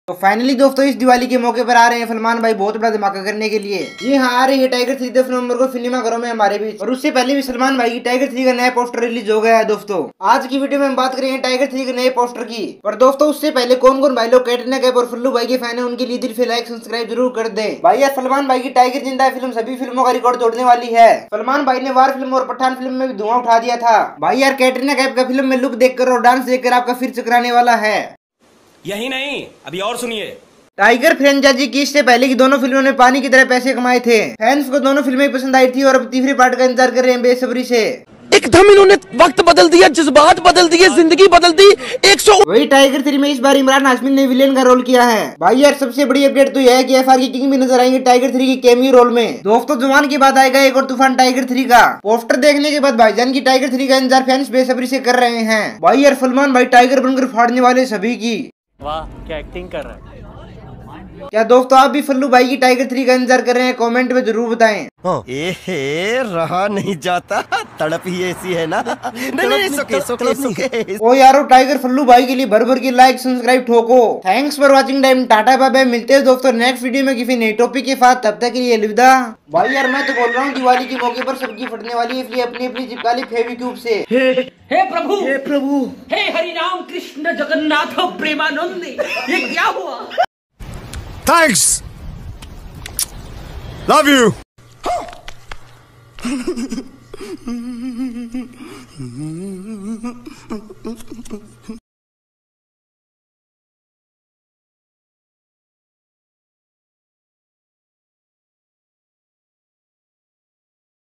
Tiger 3 Poster Teaser REVIEW | Tiger 3 Poster Reaction
Tiger 3 Poster Teaser Review Tiger 3 Announcement Featuring Salman Khan & Katrina Kaif will Open Doors to YRF Spy Universe which will later reveal Pathaan Shahrukh Khan & Hrithik Roshan in War. Tiger 3 Teaser Reaction & Full Movie Expectations would be shared in this Video. Perfect Comeback for Salman Khan & Jawan Shahrukh Khan Trailer Advantage Strategy would be unbeatable? Do leave your Public Review in the Comments Section.